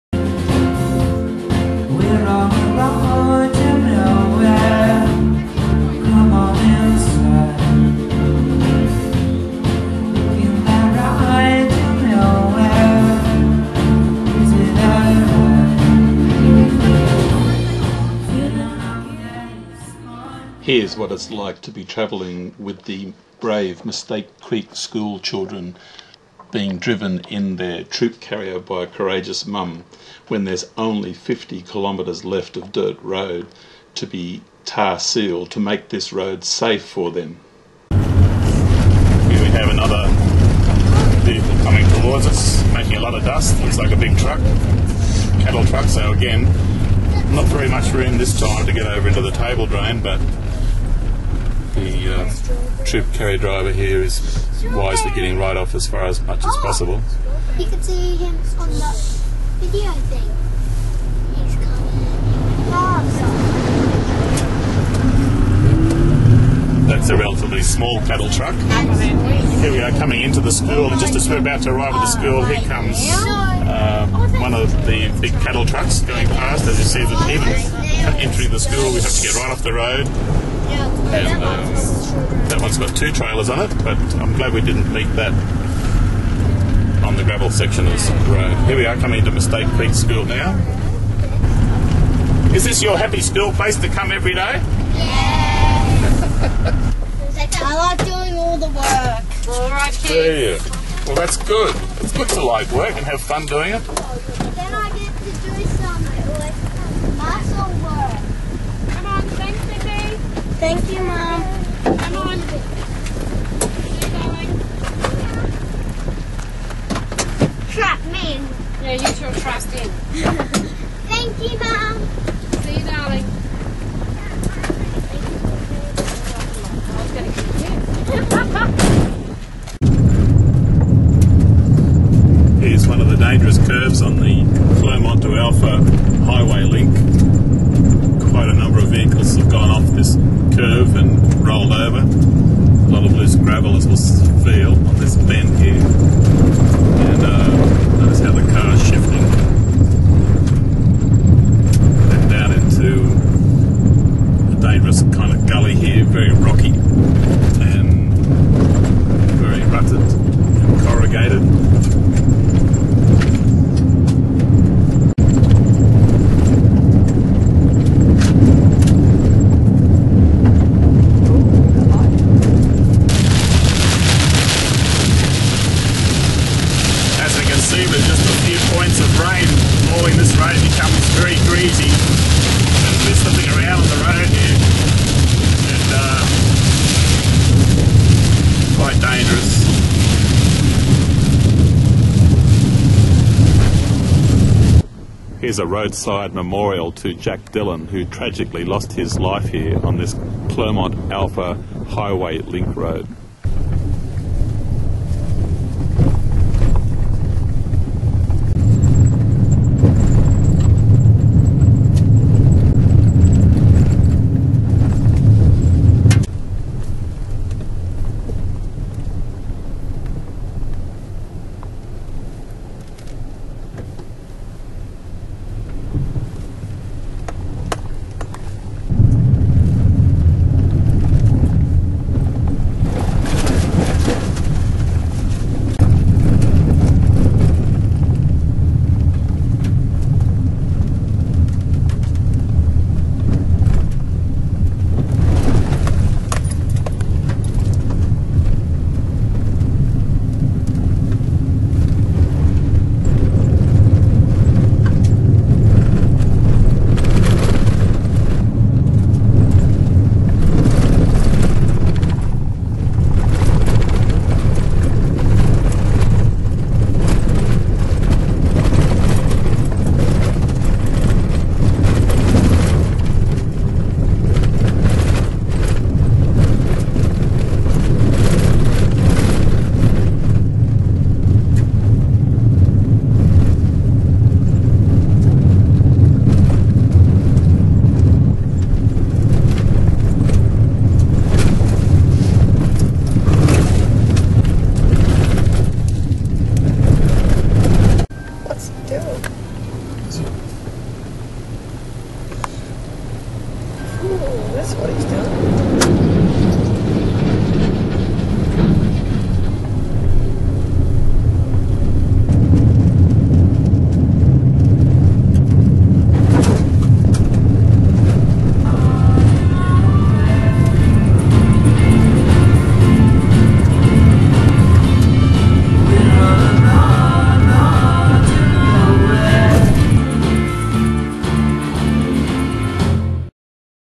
Spooky Worst Highway Link in Australia
The Clermont-Alpha HIghway Link road in Queensland, is a daily death trap for the brave Mistake Creek School children and their courageous Troop Carrier driver Mums.
We're not talking about politics here.
We are talking about the precious lives of our school children, who have to travel over 50kms on this road twice daily, just to get to and from school!
But, does the Queensland Government care?